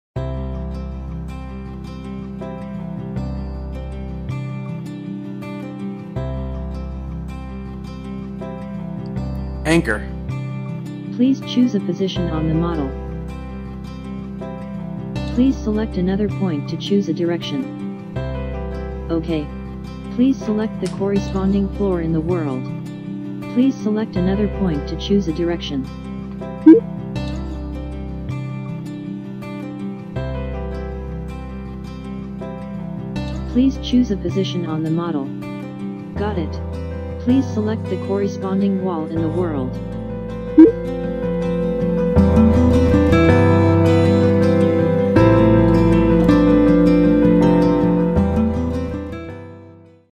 11 - Manually Anchoring Your Model
Learn how to manually anchor your Fuzor HoloLens model to your site.